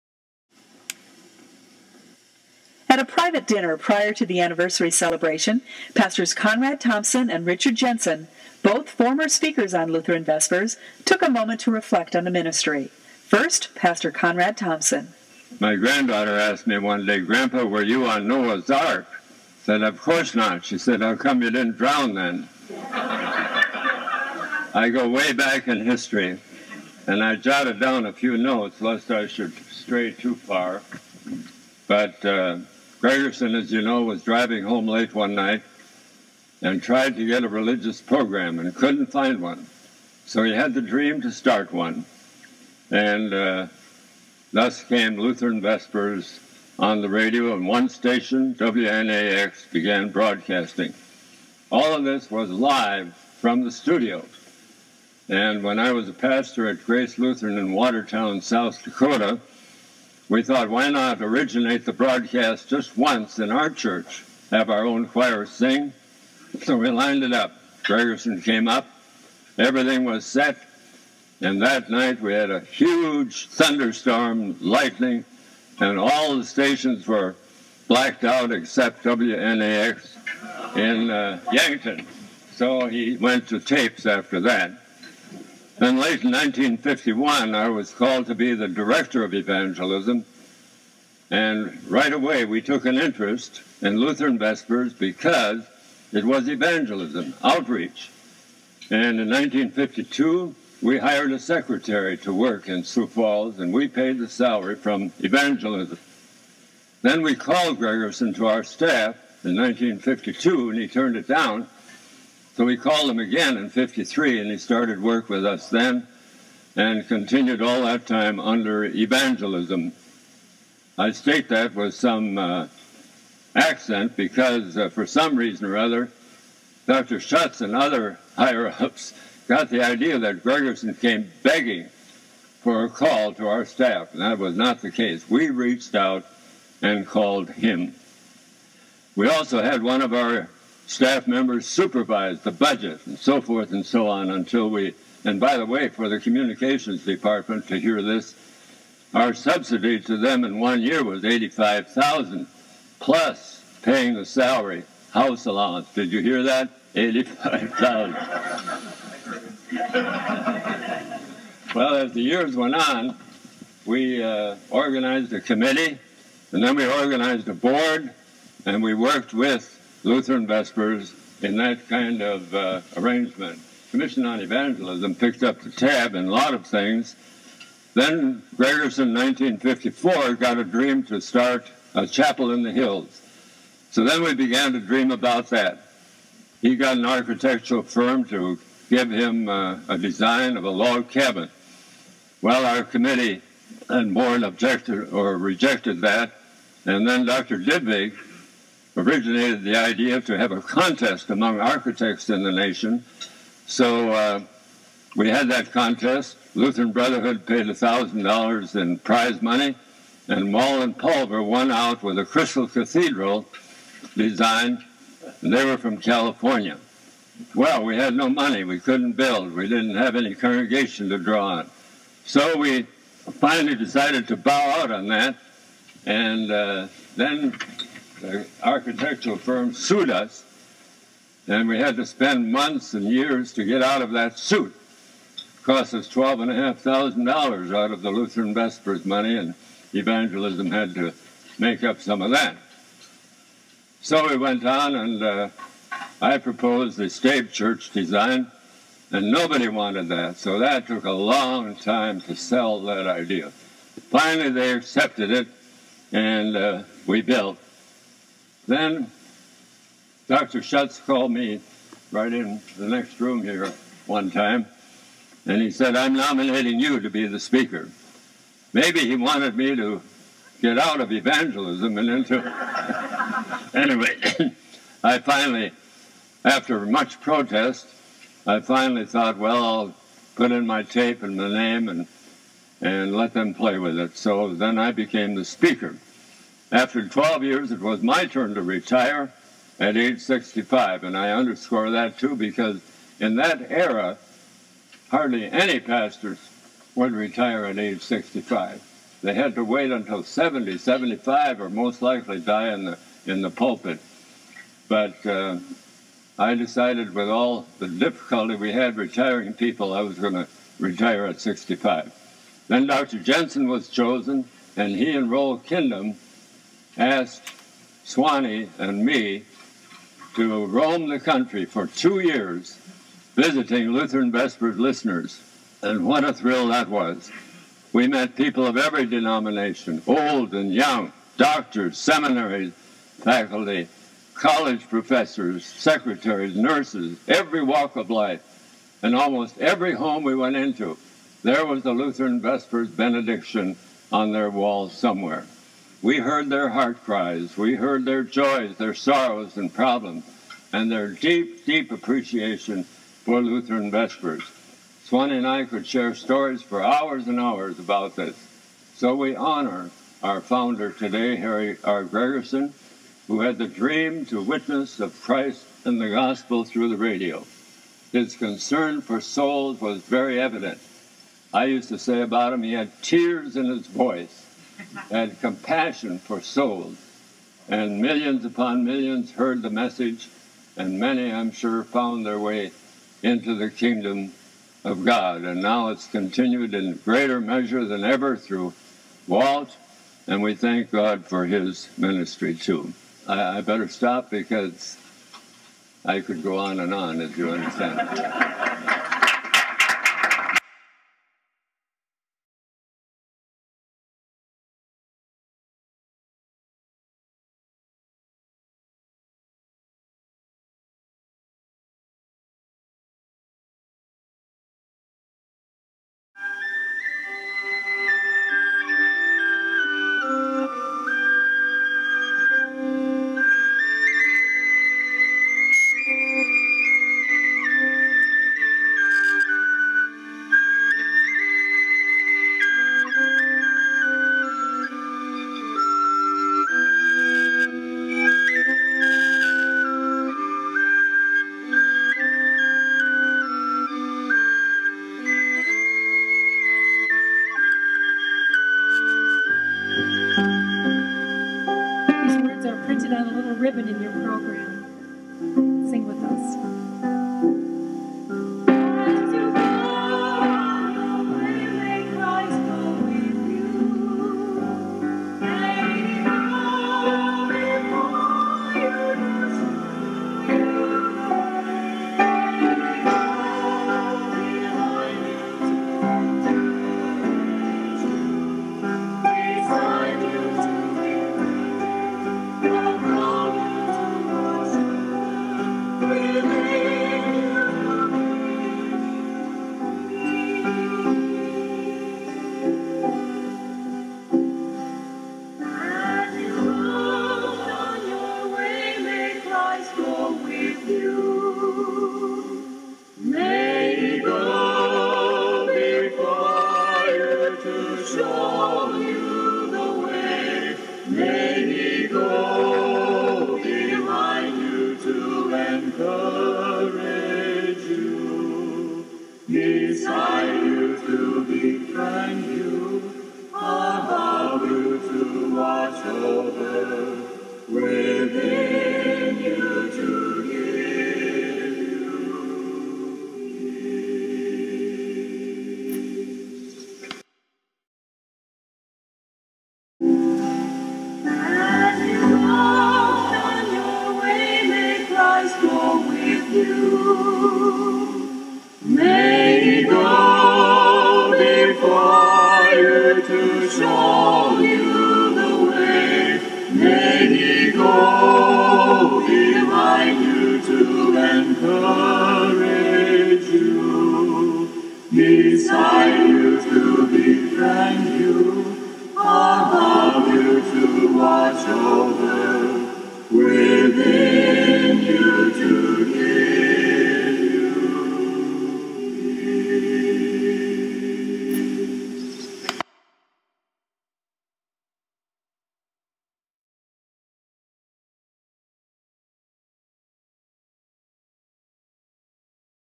Rev. Conrad Thompson, 12 years after his death: A Tribute
The video is a different kind of tribute showing the true humility of my dad as he praises the founder of Lutheran Vespers and downplays his role in the creation of the Chapel of the Hills, Rapid City, and the work he did as Director of Evangelism, Lutheran Vespers speaker and ambassador for Lutheran Vespers.  The video ends with the benediction that he wrote.